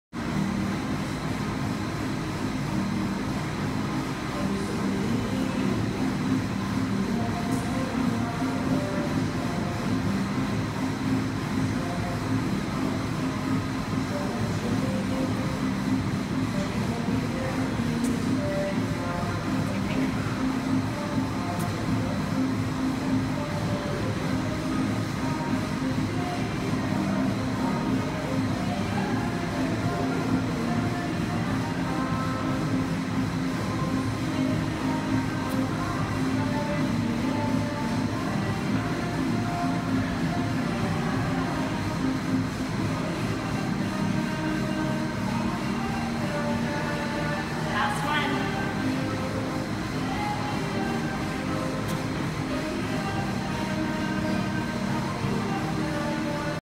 Farmer's Carry Workout Dumbbells & Kettlebells - part 2
Here it a short circuit for total body including Farmer's Carry variations with Dumbbells & Kettlebells.

What muscles does a Farmer's Carry Work?

Try a single and double kettlebell or dumbbell walk to train your entire body, get stronger and lose body fat.